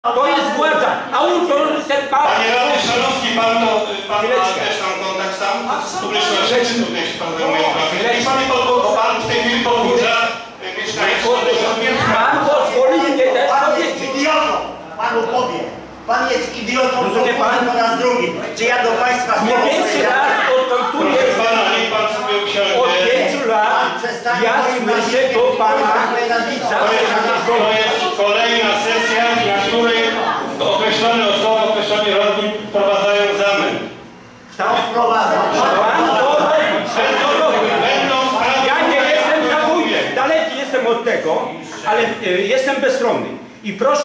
Pan jest idiotą
wściekły Jerzy Chrzanowski nazywa idiotą Przewodniczącego Rady Gminy Jedlnia Letnisko Tadeusza Kuropieskę